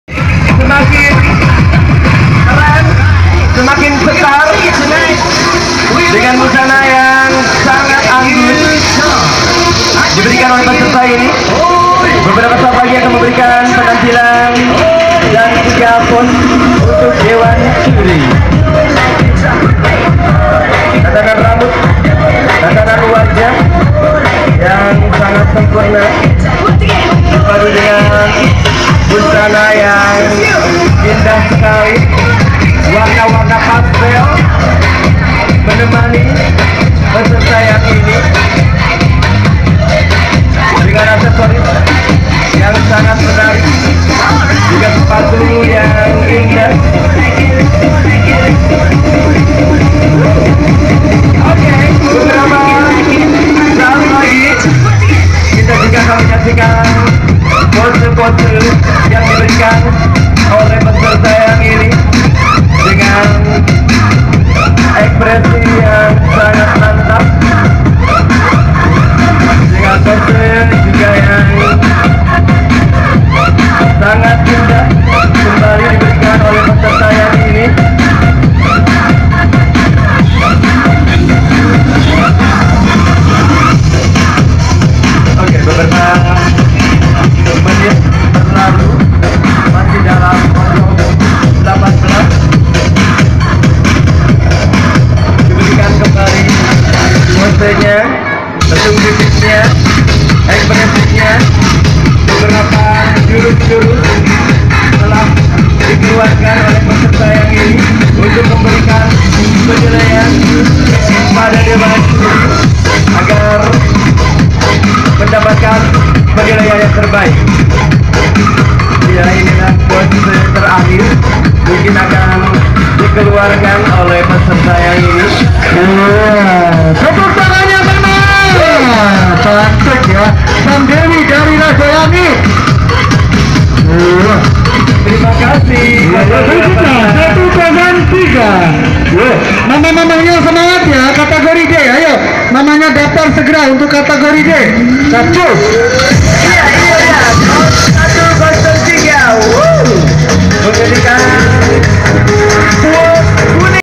Gita Banun - Duta Batik Cianjur 2016
Gita Banun - Duta Batik Cianjur 2016. Ramayana Cianjur 1 Mei 2016.